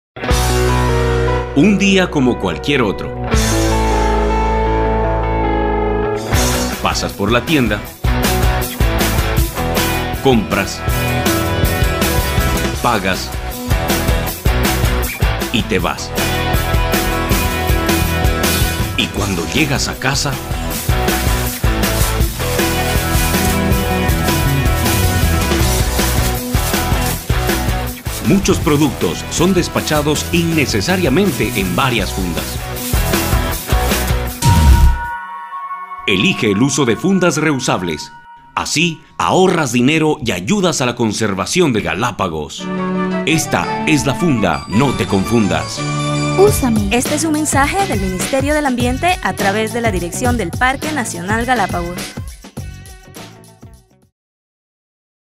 Esta es la Funda no te confundas #1
Campaña para reducir en consumo de fundas plásticas tipo camiseta en las Islas Galápagos.